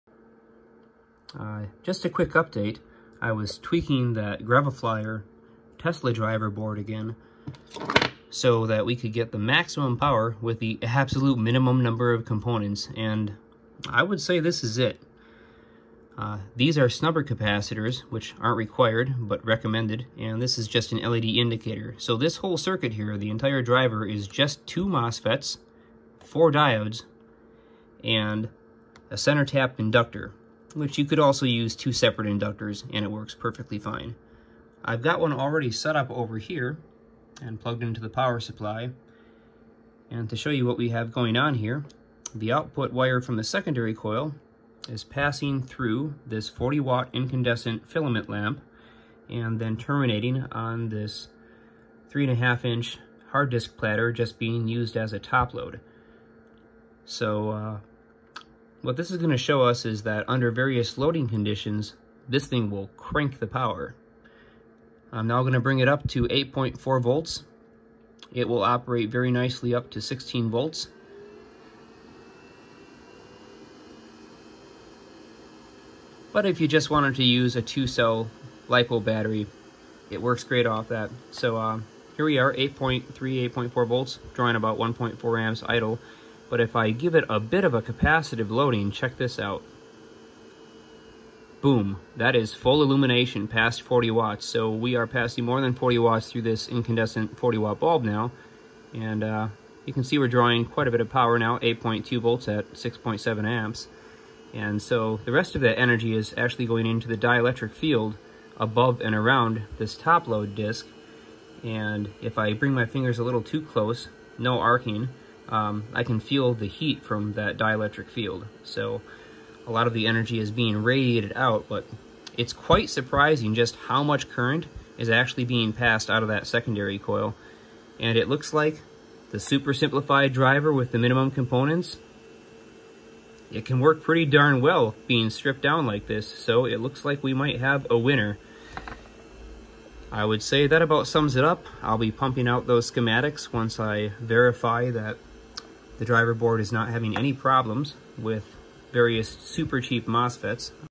The Simplest High Power SSTC? 40 W Incandescent Lightbulb Fully Lit!
I wasn’t planning to make a video on this test, but seeing that Bulb nearly burn out was just too cool.

Looks like everything is working nicely so schematics and component list are on the way soon.